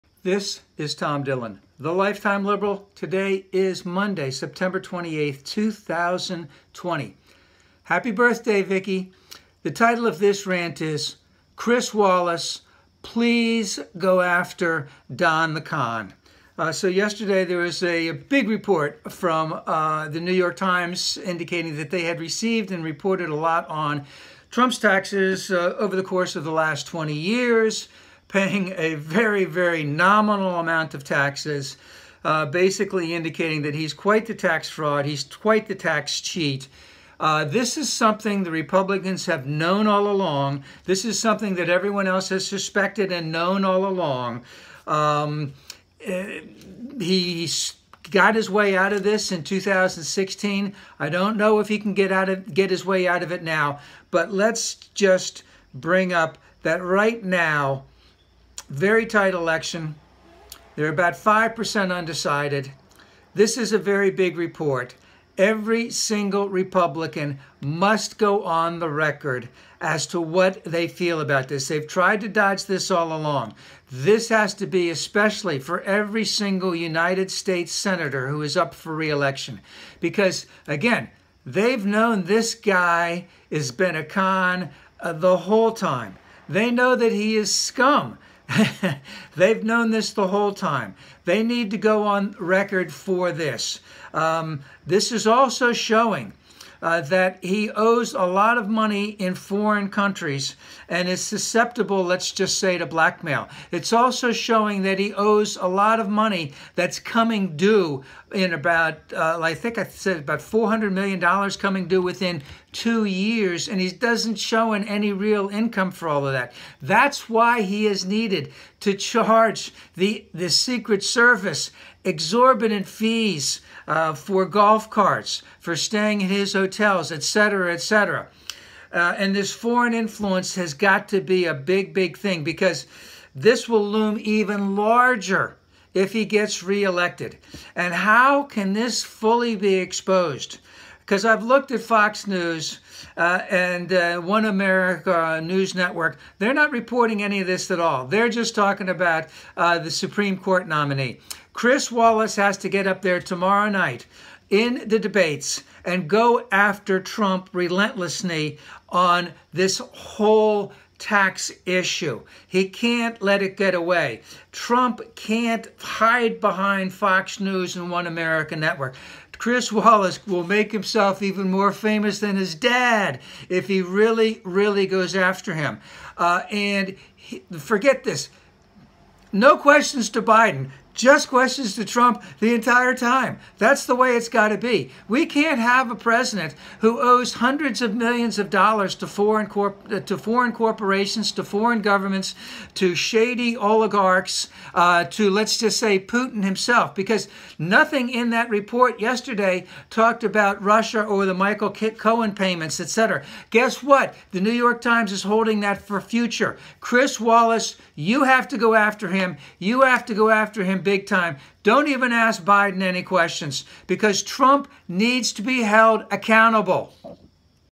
Chris Wallace, please relentlessly go after Don the Con, Public Enemy #1!
The NYT’s finally got hold of Trump’s taxes and proved what we knew all along: he is a fraud and scum. Trump will try to lie his way out of it. As the moderator of tomorrow night’s debate Chris Wallace cannot give Trump any slack. The entire debate should be focused only onTrump’s taxes.